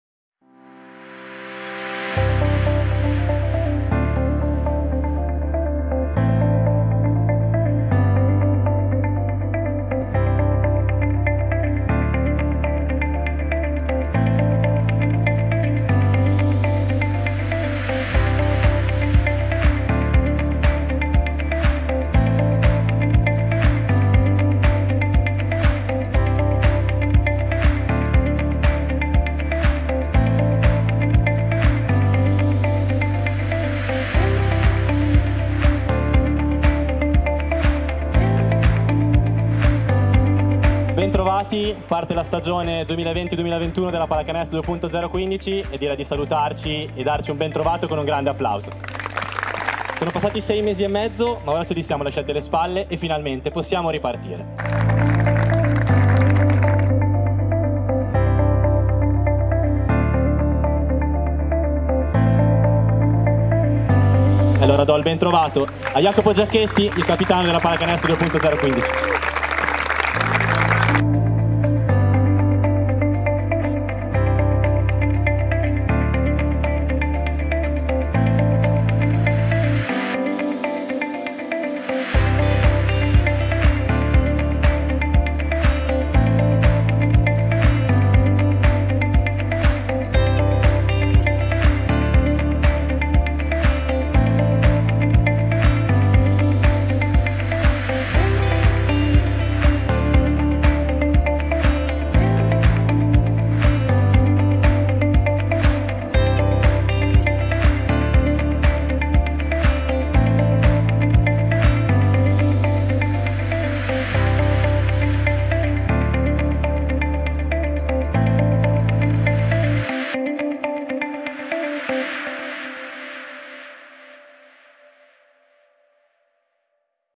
Day0: la cena al CalaFoma per inaugurare la stagione 2020/21
🎥 Riviviamo la presentazione della squadra nella splendida location del Cala FoMa: le prime parole dei biancorossi, gli applausi dei tifosi e la cena sulle rive del lago... Il modo migliore per inaugurare la stagione 2020/2021 🏖️